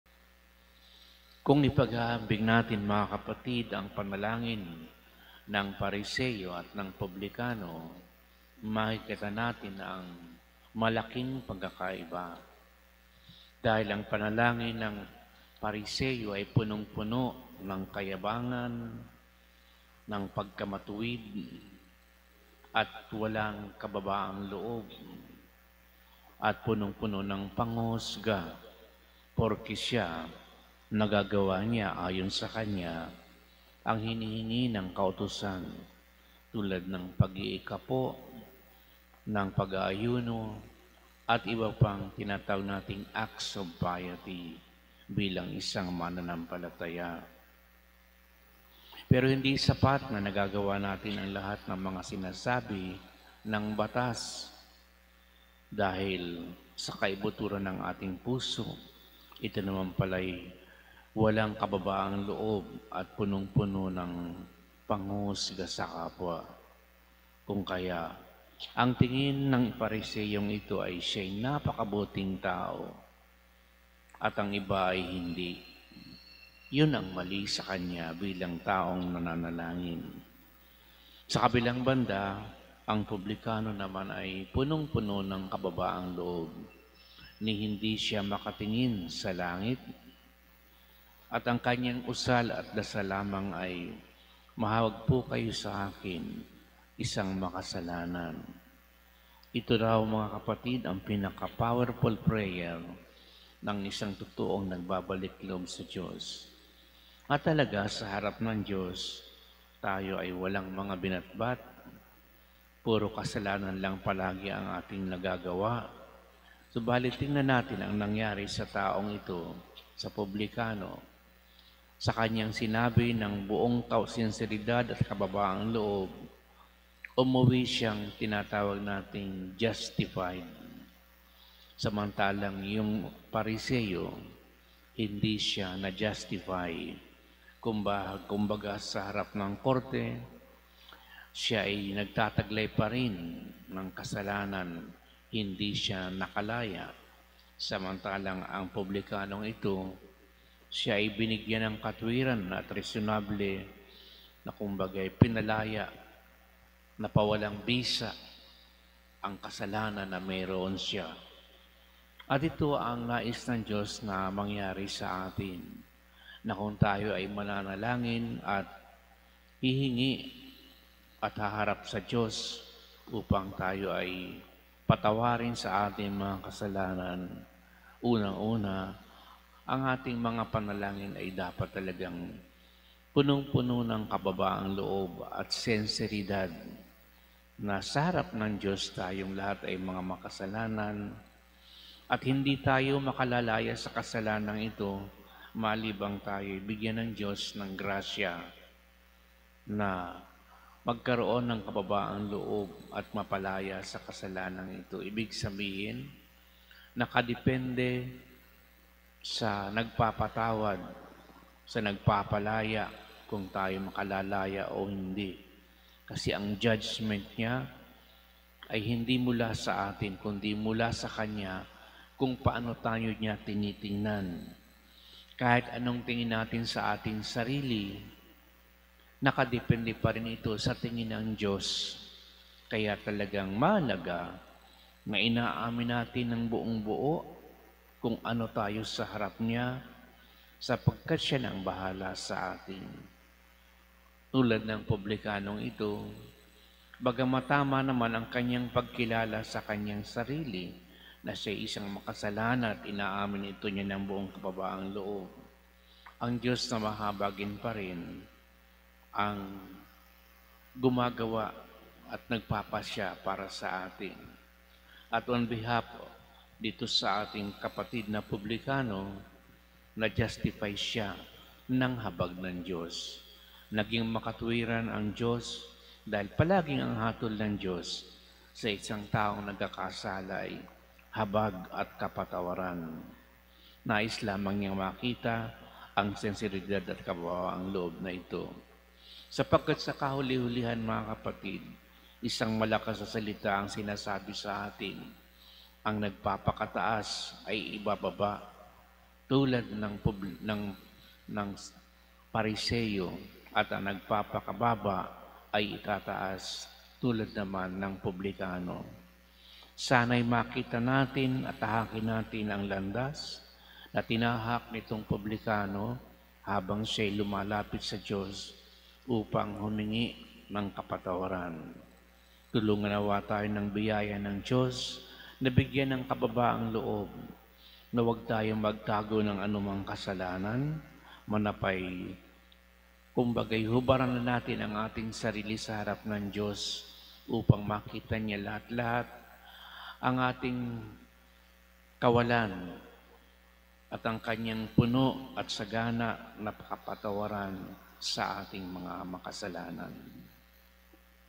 SabiNiFather - March 18, 2023 | 6 AM Mass
SabiNiFather - Homily of Fr. Efren Villanueva on the Missa Crucis (Mass of the Cross) [Saturday of the 3rd Week of Lent] - 6:00 AM Mass | March 18, 2023

Jesu Christi Passio sit semper in cordibus nostris (May the Passion of Jesus Christ be always in our hearts.)